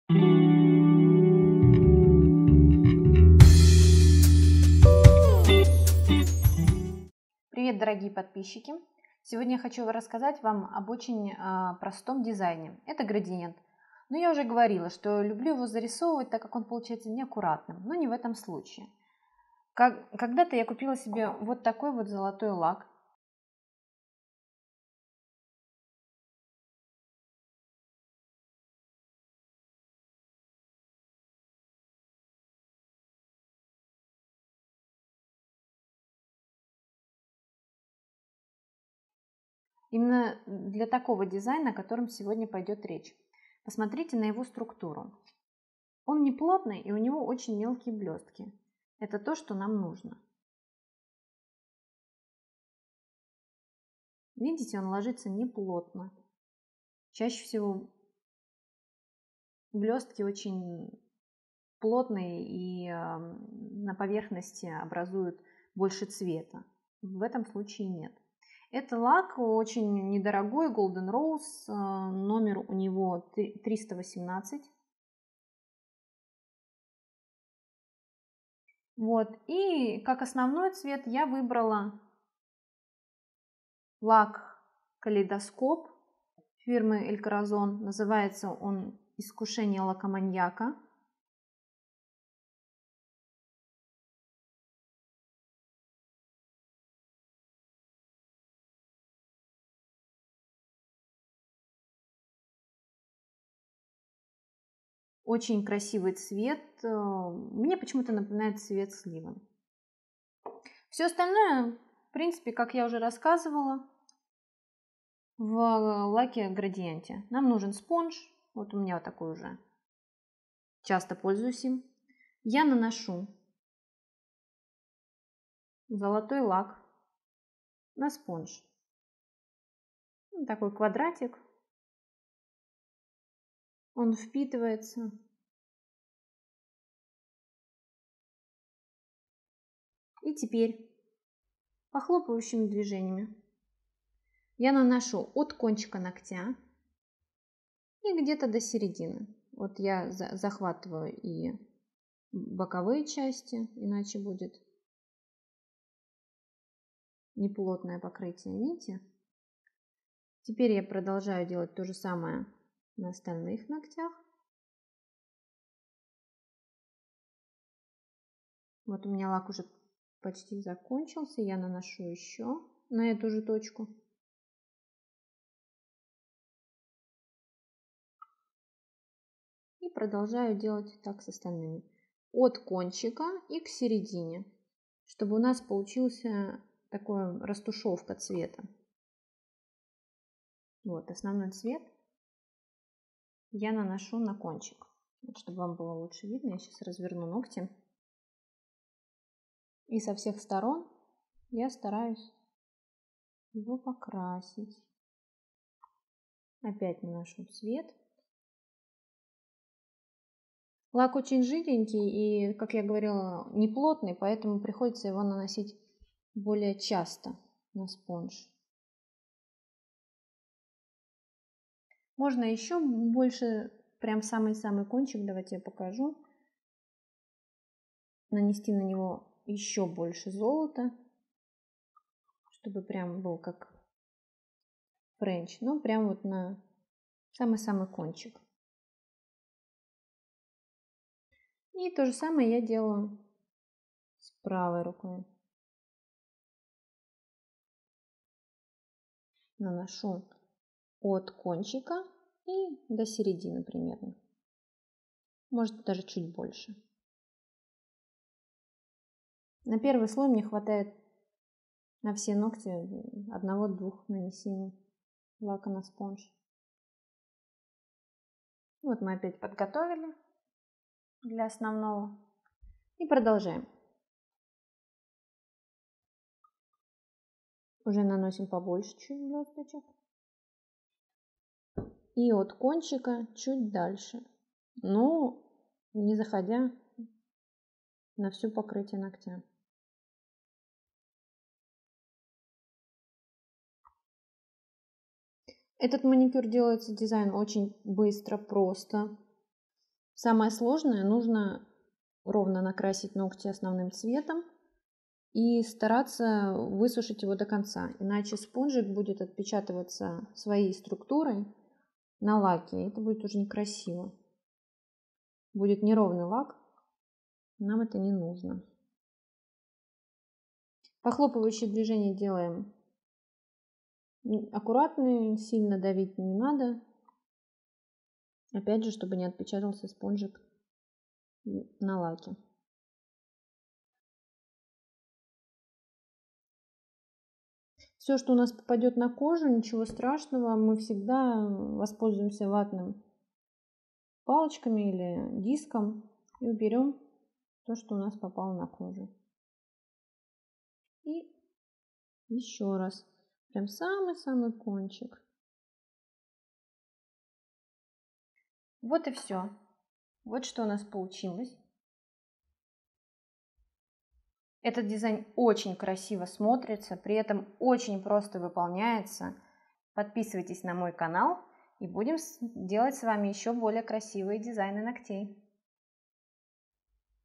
Урок дизайна ногтей - Золотой градиент
Мне очень нравится делать градиент. Но он получается не таким уж идеальным. Плавный переход очень трудно сделать. Но я нашла свой вариант. Сделала градиент с помощью золотого лака и на ногтях он смотрится очень аккуратно. Сделать такой дизайн ногтей сможет любая модница, ничего сложного в нем нет.

Другие уроки, а также свотчи лаков вы сможете найти на моем канале, сайте и группе Вконтакте